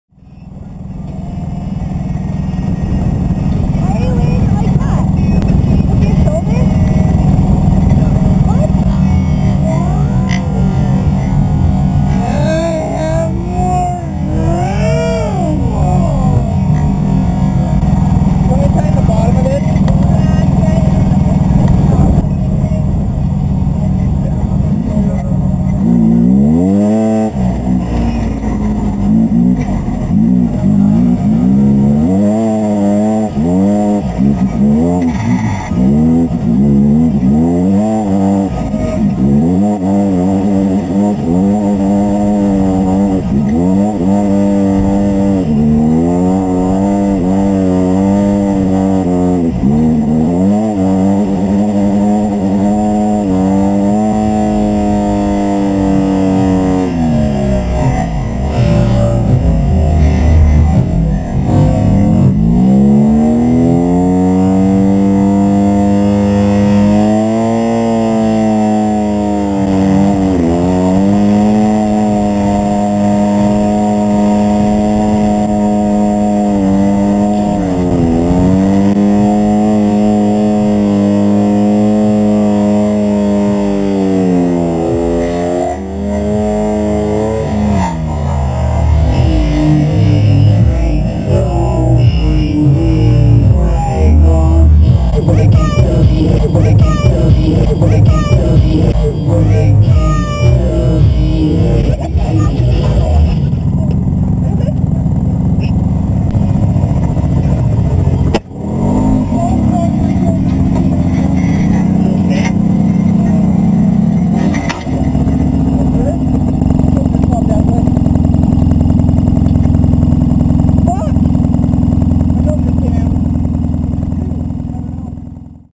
Rzr Tip at Sand Hollow!
Dj tipping the rzr over within 5 minutes of riding, which somehow ended with a dislocated shoulder.